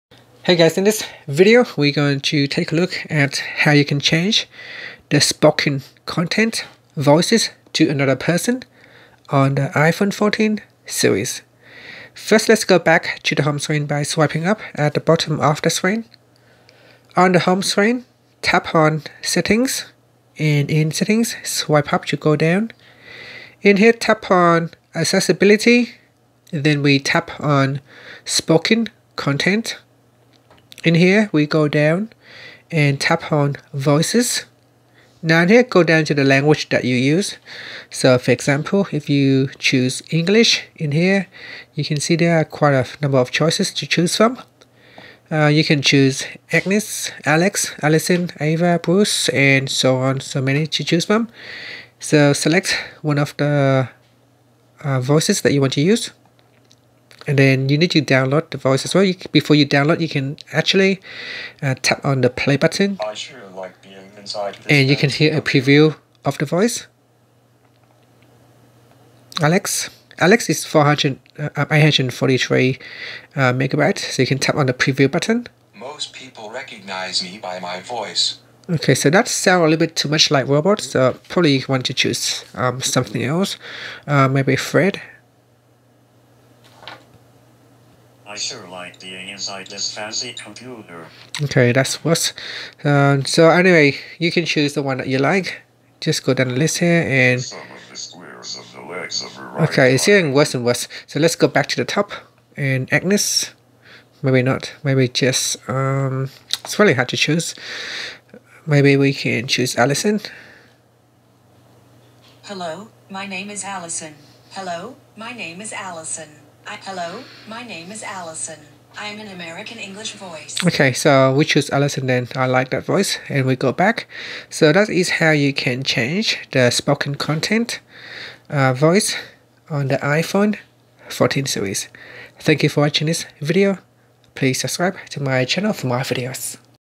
iPhone 14's/14 Pro Max: How to Change Spoken Content Voices to Another Person
Learn how you can change the Spoken Content Voices to another person on the iPhone 14/14 Pro/14 Pro Max/Plus.